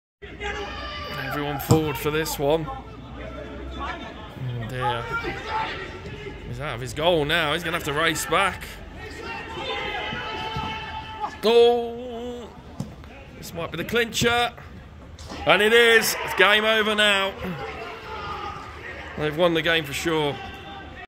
Ball Haye Green Vs Redgate Clayton
3rd goal for Redgate Clayton in their 3-1 win over Ball Haye Green